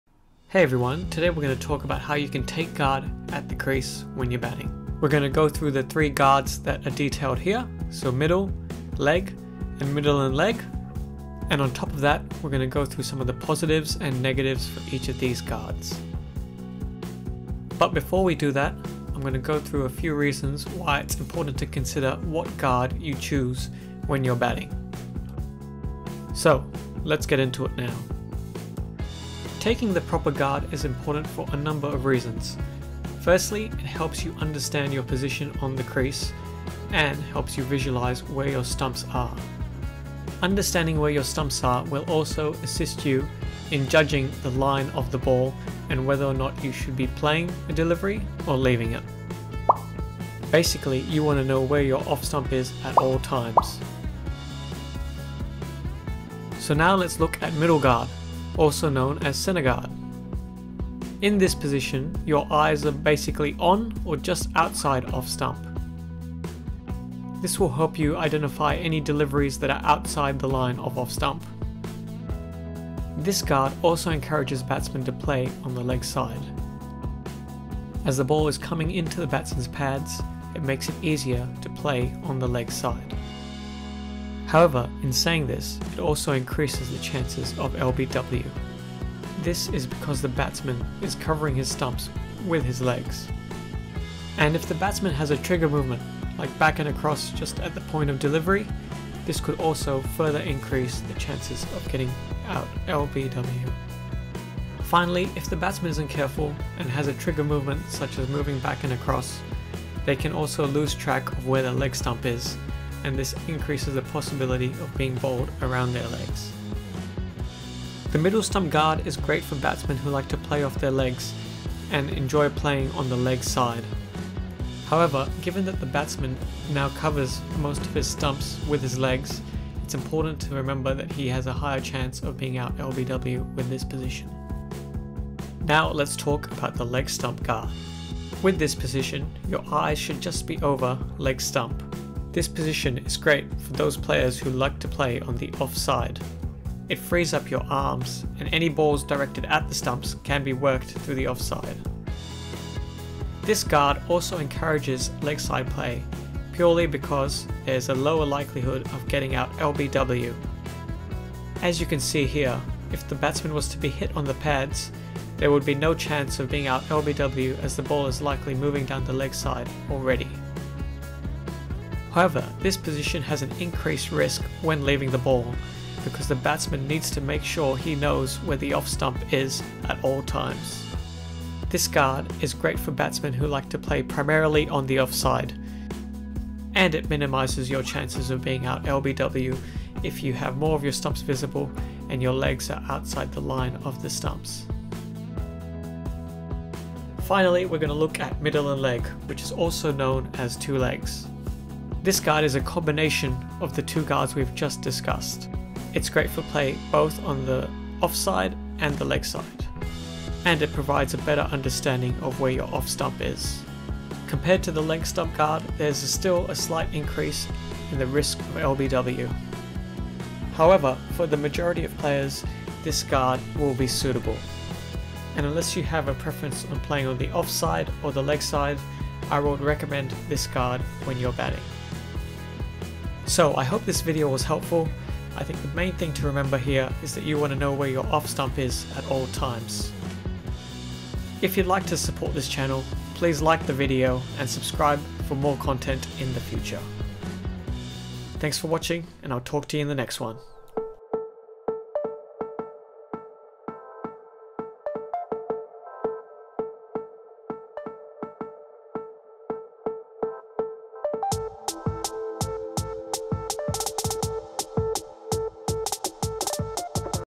How To Choose A Guard In Cricket? Middle Or Leg? Cricket Batting Tips - How To Take The Right Guard
Unlock your cricket potential with my cricket journal & planners! 🏏

We are going to discuss how you should take your guard for cricket. Learn about the batting stance and what batting guard you should choose when batting. How you can choose the batting guard most suitable for you. The batting guard is one of the most important elements of your batting preparation. Learn why the batting guard is so important and why you need to consider which guard will compliment your style of play.

In this video we discuss the three different types of batting guard, middle, leg and two-legs, along with going through the positives and negatives of each guard.

This video will help you decide what guard you should experiment with and one that will help you play to your fullest potential.

🏆 Take your game to the next level!
Invest in your success with my PRO Cricket Journal today!

0:00 Introduction To All Three Batting Guards
0:27 Importance Of Taking Guard
0:58 Middle Stump Guard
2:16 Leg Stump Guard
3:16 Middle & Leg
3:54 Summary